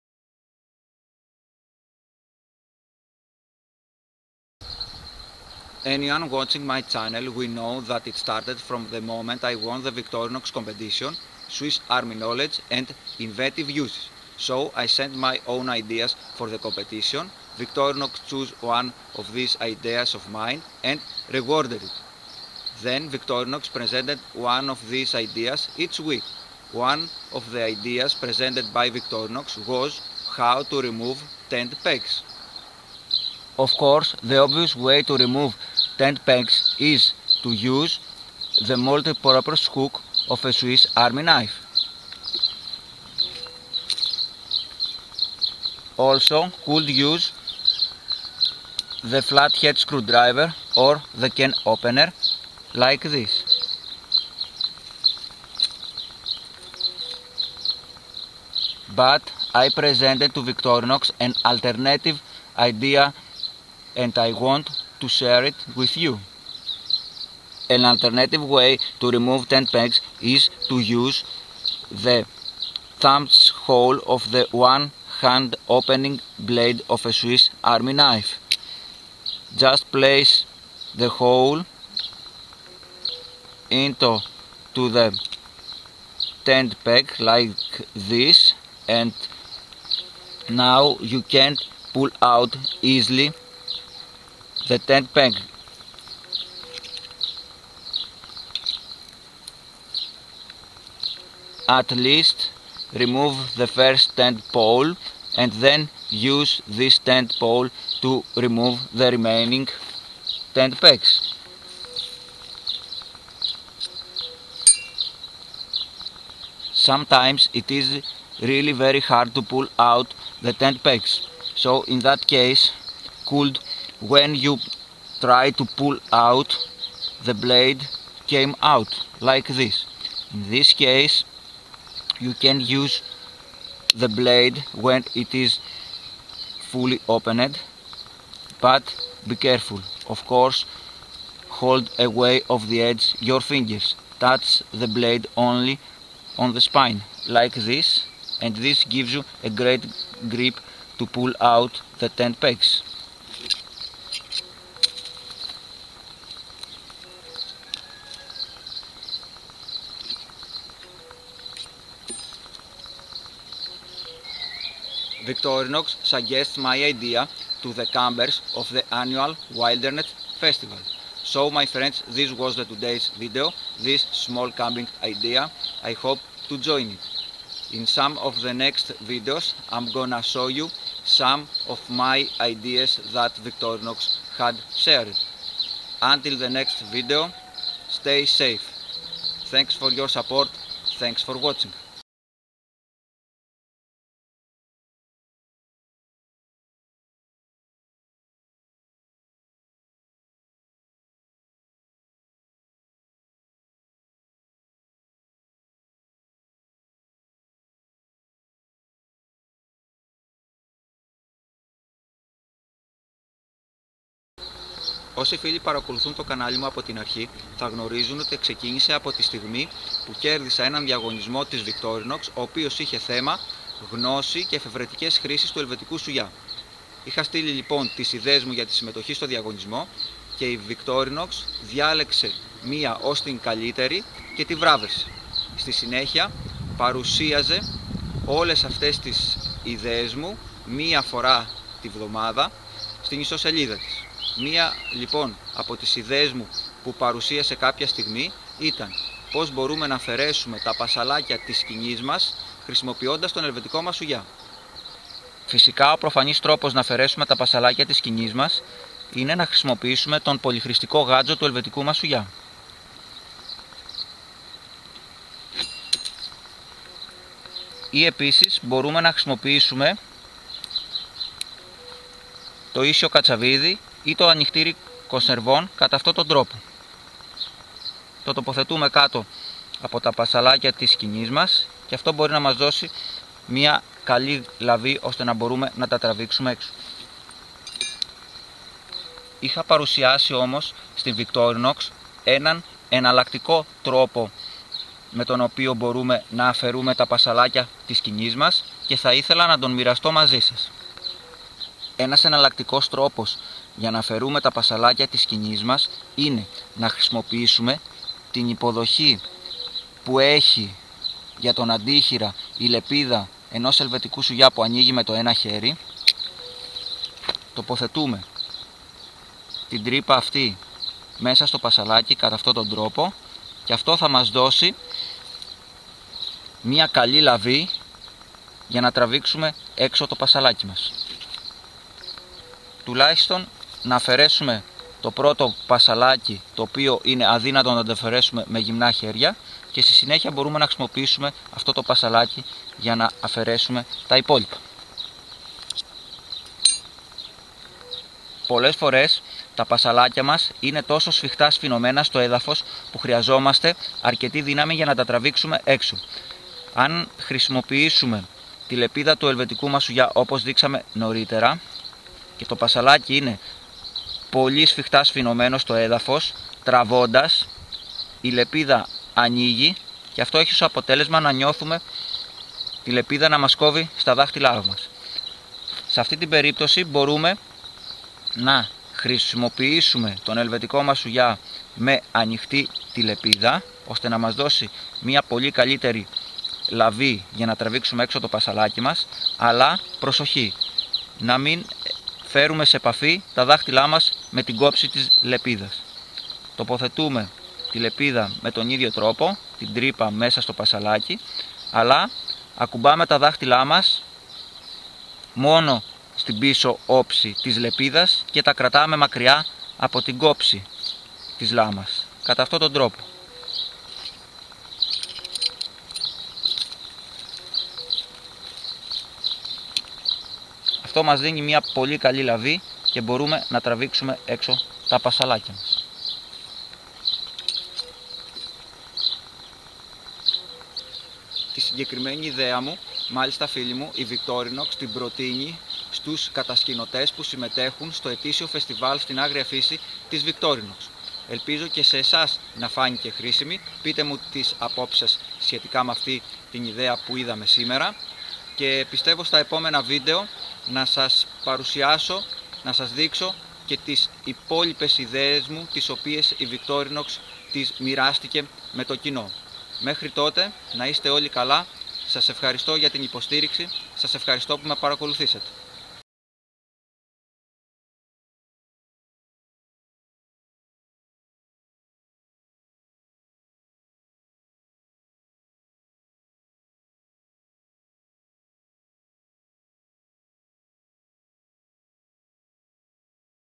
Removing Tent Pegs swiss army knife.Αφαιρεση Πασσαλων Σκηνης με Ελβετικο Σουγια.
English Language 00:00 Minute / ΕΛΛΗΝΙΚΑ 3:30 Λεπτο.
Camping Hack:How to remove tent pegs using a swiss army knife.The video from the start,until the 03:30 minute,is in English language.
Πως μπορουμε να αφαιρεσουμε τα πασσαλακια της σκηνης μας,χρησιμοποιωντας εναν ελβετικο σουγια.Το βιντεο απο το 3:30 λεπτο,μεχρι το τελος,ειναι στα ΕΛΛΗΝΙΚΑ.